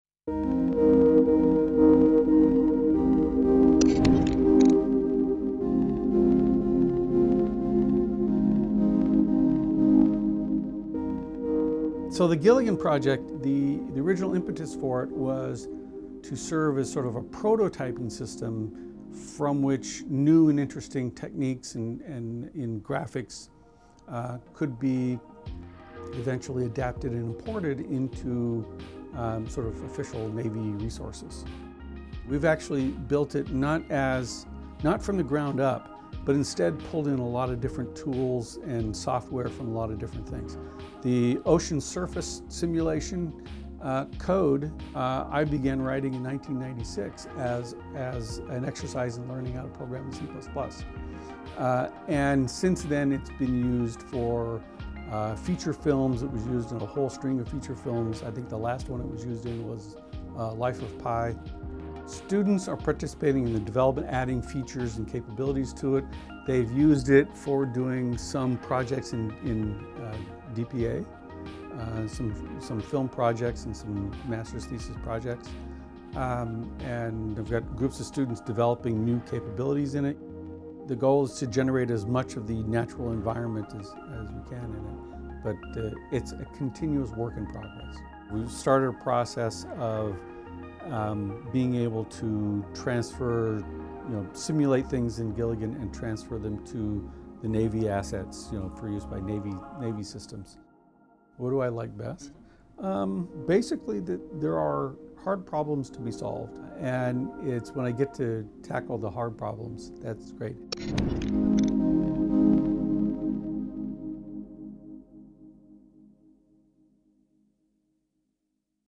Gilligan Project
Jerry Tessendorf, a professor of visual computing, and his students have developed a simulator, Gilligan, they use for teaching, research and film production. The kind of images created with Gilligan not only help breathe life into movies and video games. Tessendorf and his team have also started applying Gilligan to engineering and remote-sensing challenges, especially those faced by the U.S. Navy.